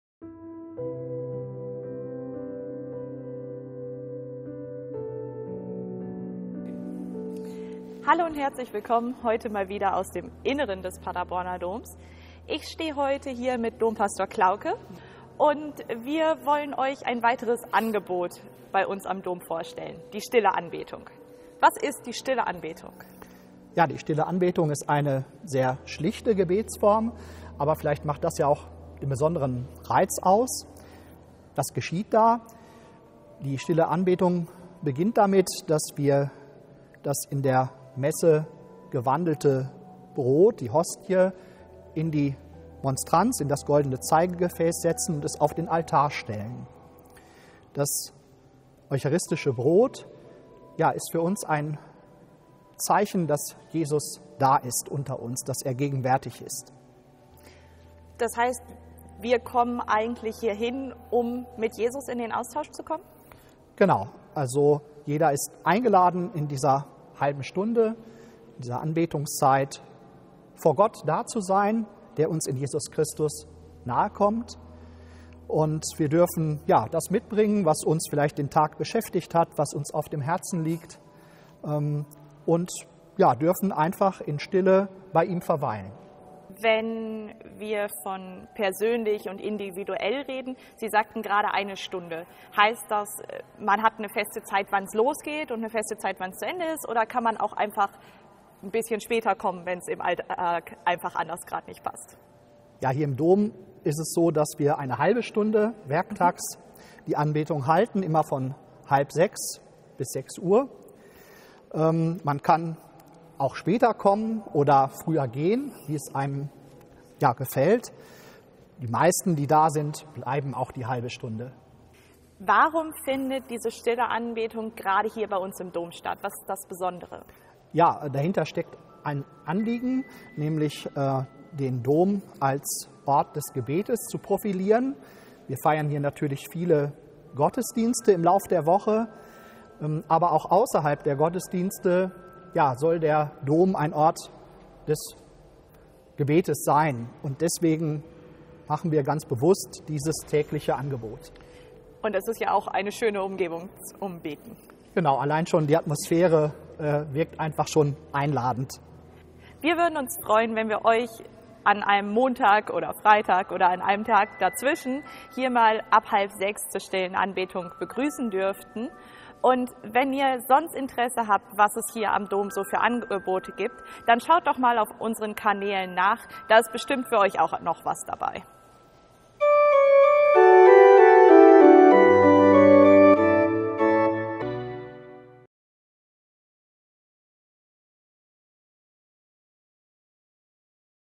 Die Stille Anbetung im Paderborner Dom
Jeden Werktag um 17.30 Uhr wird im Dom das Allerheiligste auf den Altar gestellt. Es ist der Beginn der Stillen Anbetung. Kira Lietmann und Dompastor Matthias Klauke erklären euch in diesem Video was es damit auf sich hat.